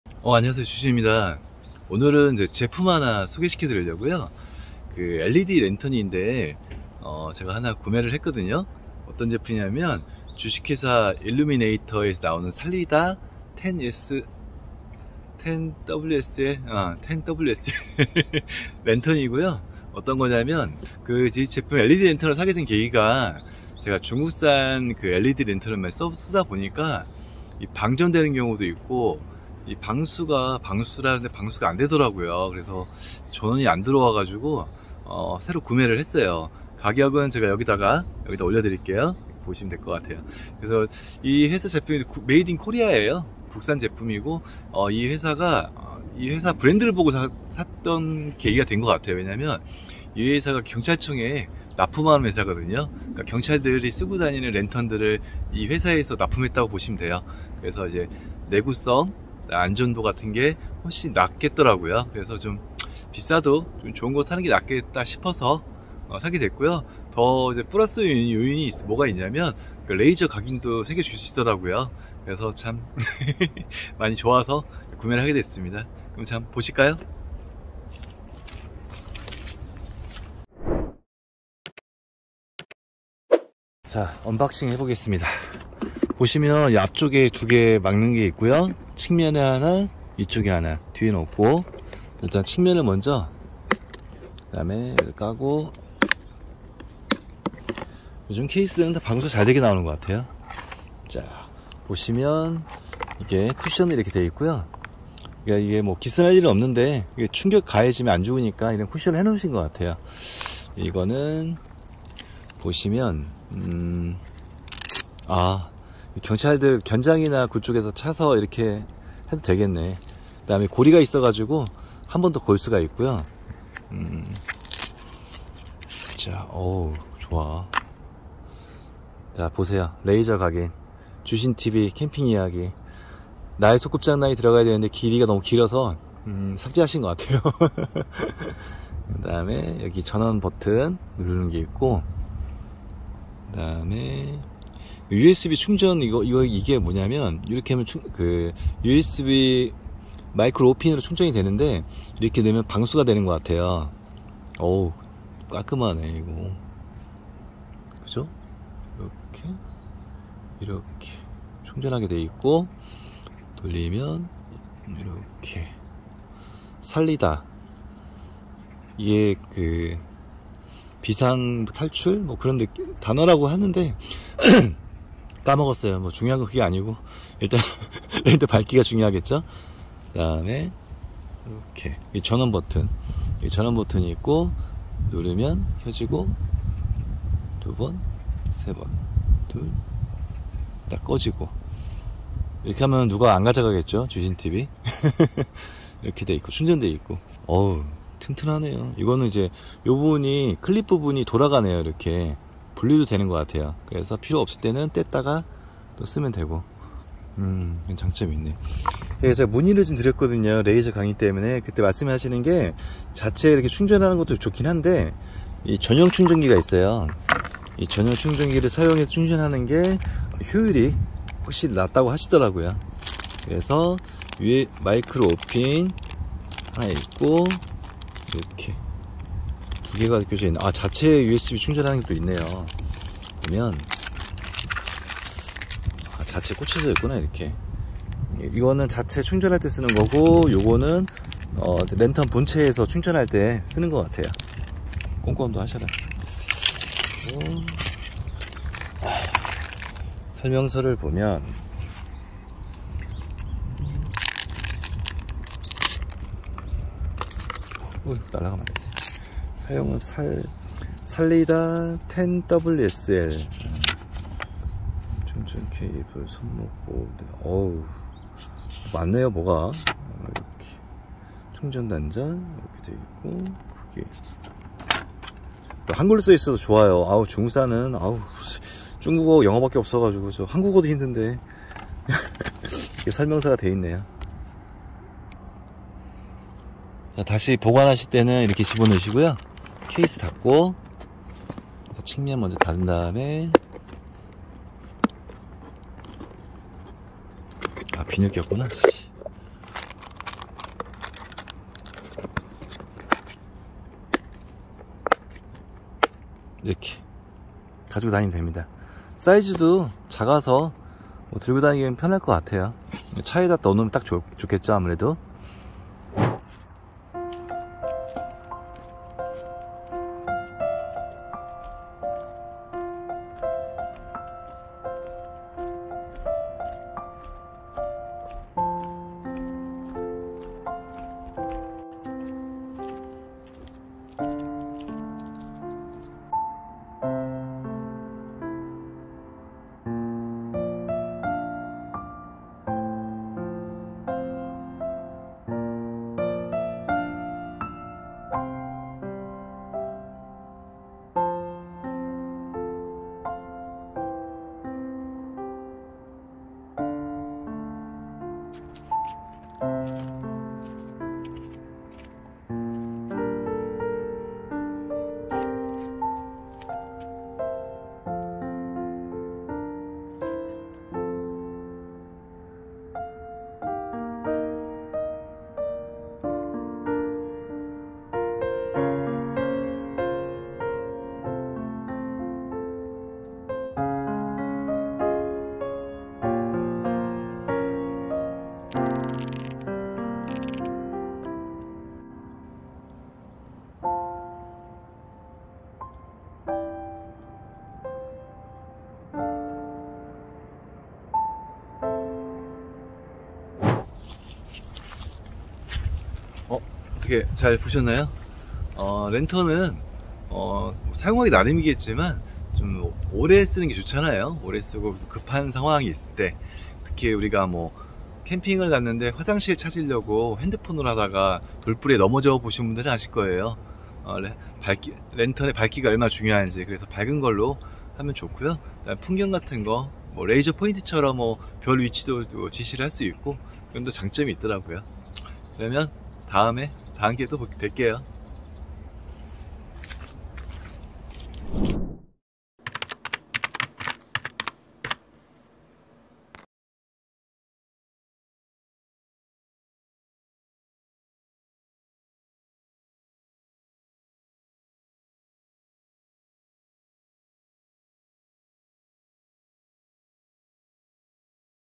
백패킹용품/(주)일루미네이터 /국산손전등/ salida 10WSL
쥬신TV 입니다

오늘은 led손전등 소개를 해드리려고 합니다
(주)일루미네이터 라는 salida 10WSL 제품이구요
백패킹 이나 캠핑을할때 꼭필요한 제품이죠